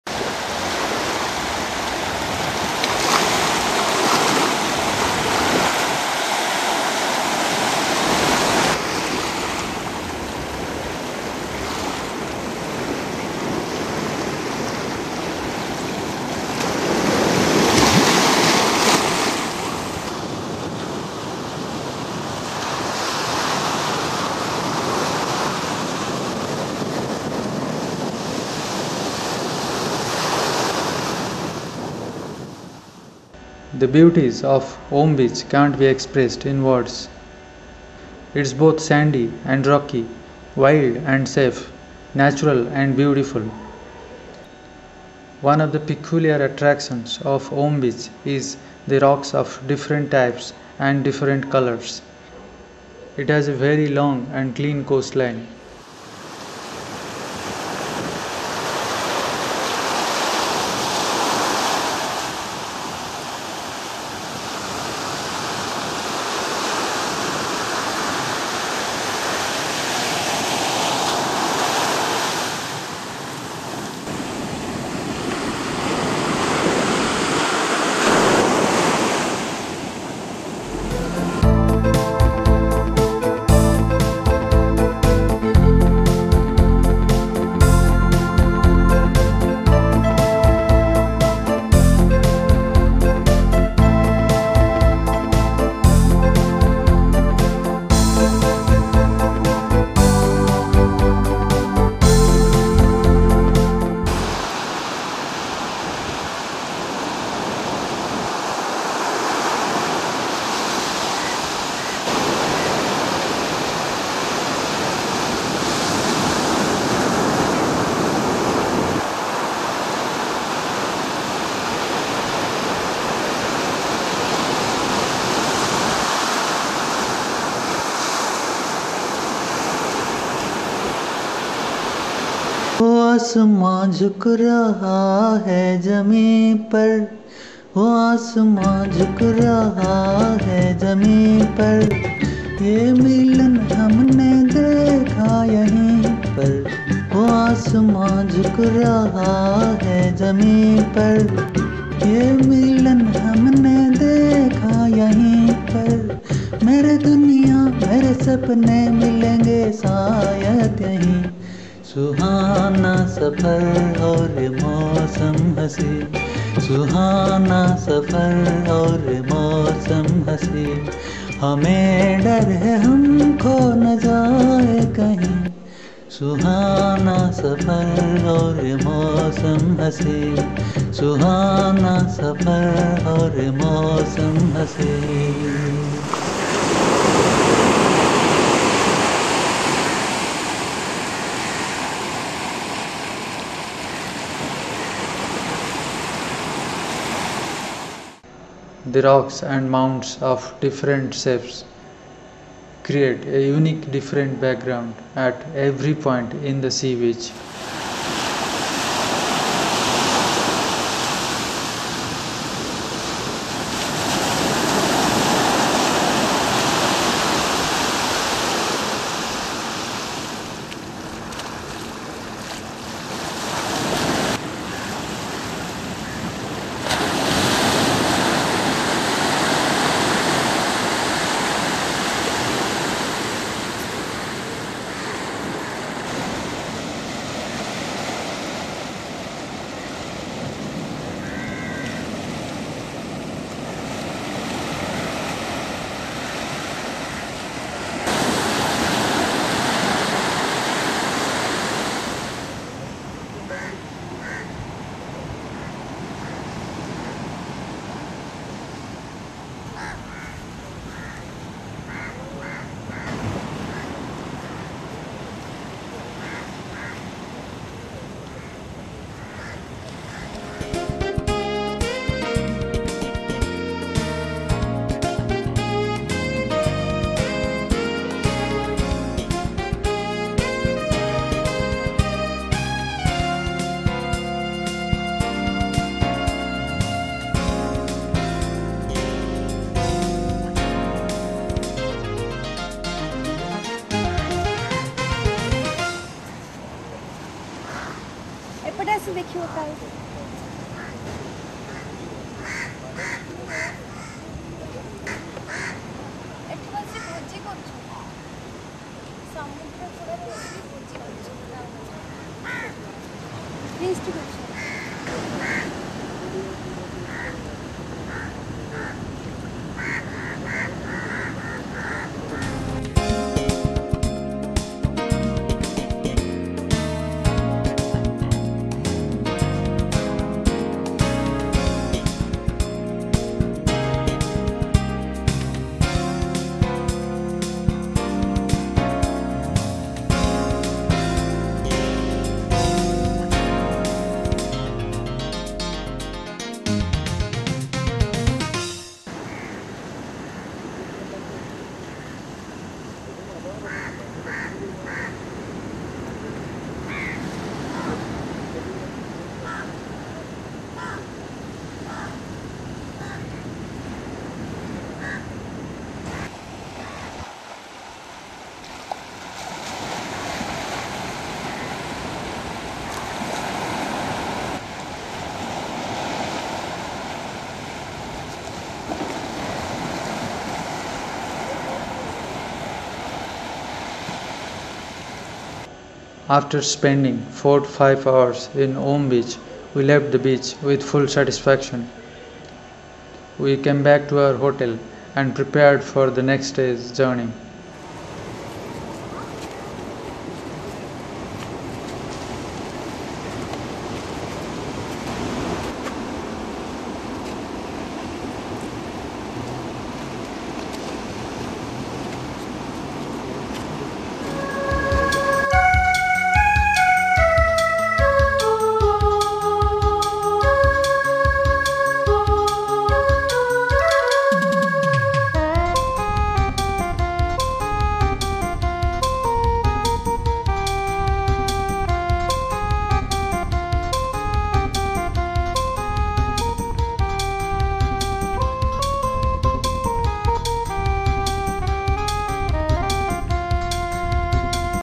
Om Beach- the Best Beach of Gokarna (Part2)
Om beach is another beach of Gokarna about 5 kms from Gokarna Mahabaleswar Temple. You may either go by a hill road or trek on the hills from Kudle beach. The curves of the sea beach look like a big “Om” spreading about one km. This is undoubtedly the most beautiful beach out of atleast 30 beaches I have seen.